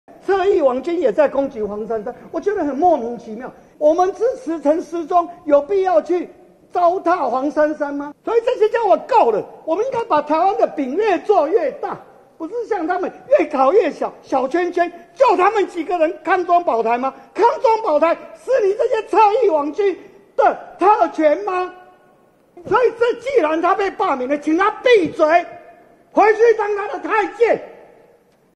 挺黃珊珊被王浩宇批開除 王世堅：閉嘴 回去當太監｜政治
堅哥小心血壓啊...